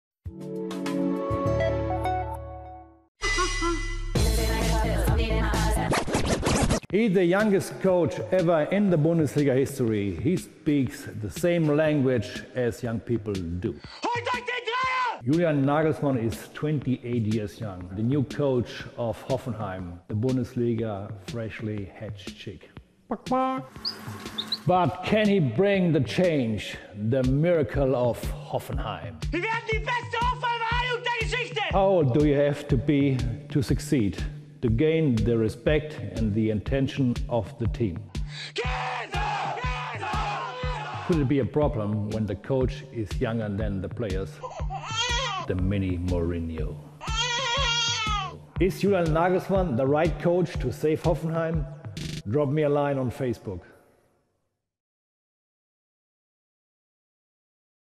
Bertholds Bundesliga #24: YOUNGEST COACH | Kick off!
Der knochenharte Weltmeister von 1990 nimmt kein Blatt vor den Mund: Hoffenheim ist Tabellenvorletzter! Jetzt kommt Julian Nagelsmann, 28 Jahre jung. Kann der jüngste Trainer der Bundesligageschichte den Verein vor dem Abstieg retten?